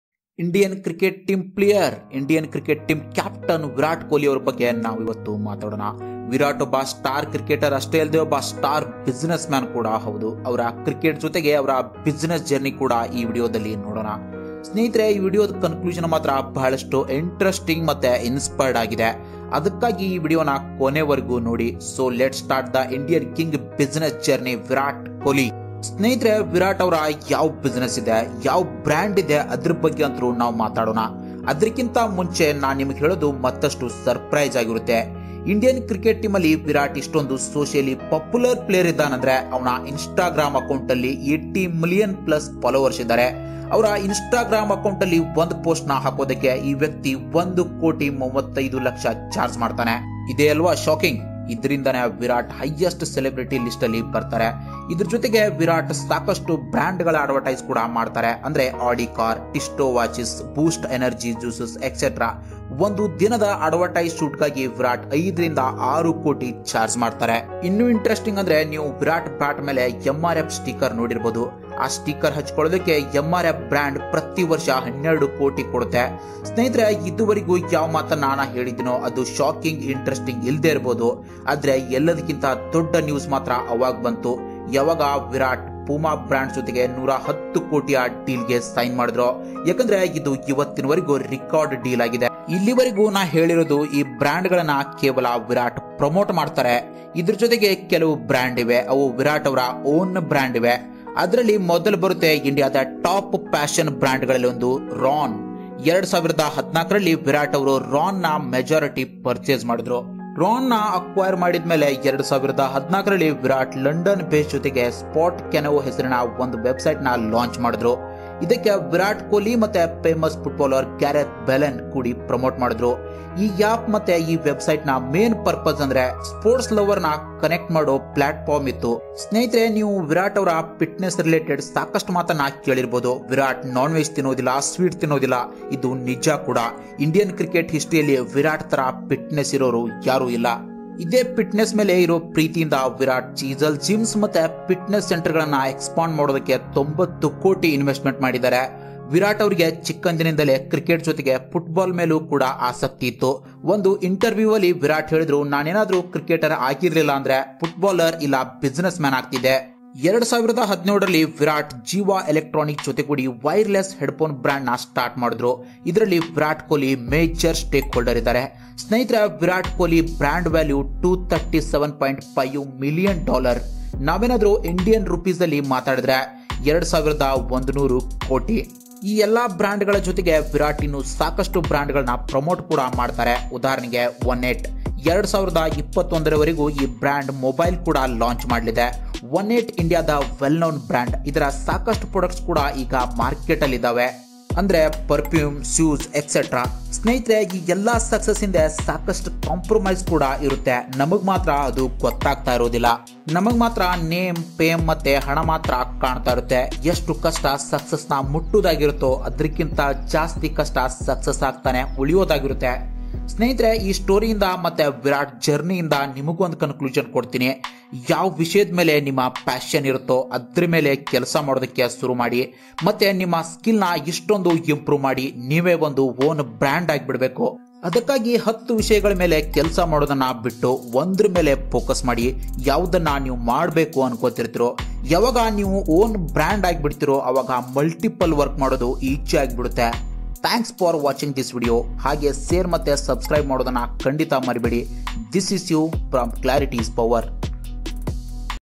Virat kohli biography/ Virat kohli business journey/ by clarity is power
virat kohli business journey

Virat kohli is one of the famous cricketer and indian captain. Many people knows his cricket journey but very few are aware of his business journey.

Brand value of virat kohli is 237.5 million dollars.

I am sure you Will get positive inspiration to from this video

All the best keep growing. !